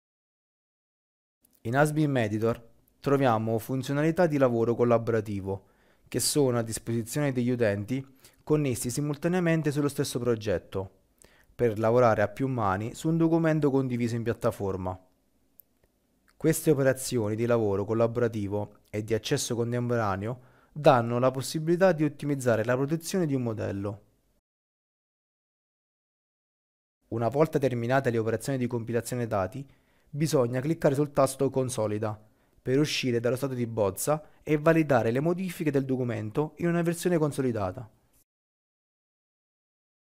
Corso completo di usBIM.editor-Lez#5 -Collaborare sullo stesso modello con i colleghi in tempo reale
Corso di formazione completo di usBIM.editor, l'IFC editor per lavorare completamente su un file IFC creato con Edificius®, Revit®, ArchiCAD® o un altro Authoring BIM